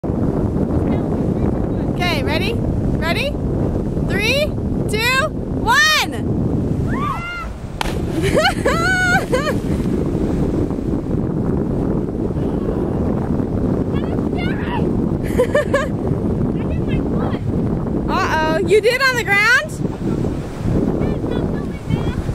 Mo cliff jumping
Monique Cliff jumping at the point in Laie